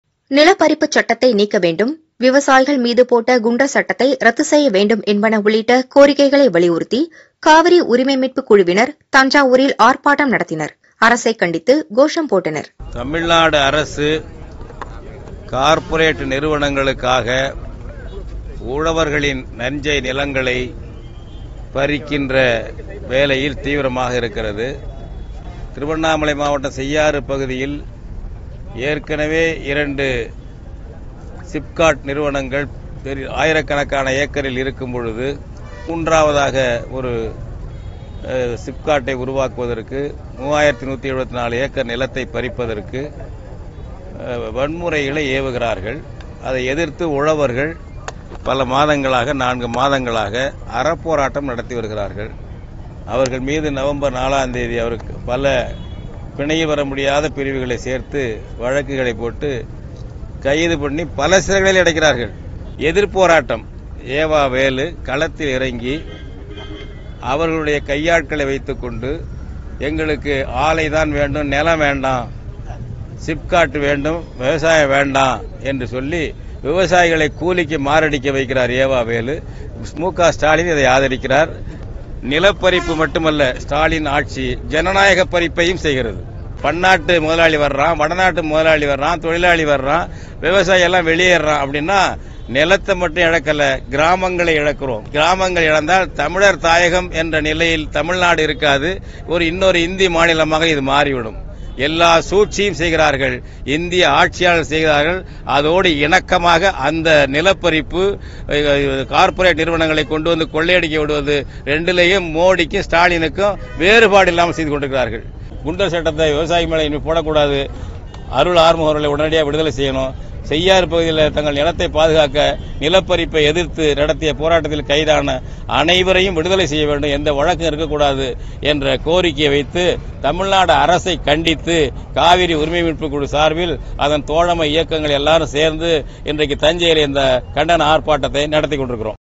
திமுக அமைச்சர் எவ வேலு மீது மணியரசன் பகிரங்க குற்றச்சாட்டு | DMK Minister E.V.Velu | Farmers Protest
நில பறிப்பு சட்டத்தை நீக்க வேண்டும்; விவசாயிகள் மீது போட்ட குண்டர் சட்டத்தை ரத்து செய்ய வேண்டும் என்பது உள்ளிட்ட கோரிக்கைகளை வலியுறுத்தி காவிரி உரிமை மீட்பு குழுவினர் தஞ்சாவூரில் ஆர்ப்பாட்டம் நடத்தினர். அரசை கண்டித்து கோஷம் போட்டனர்.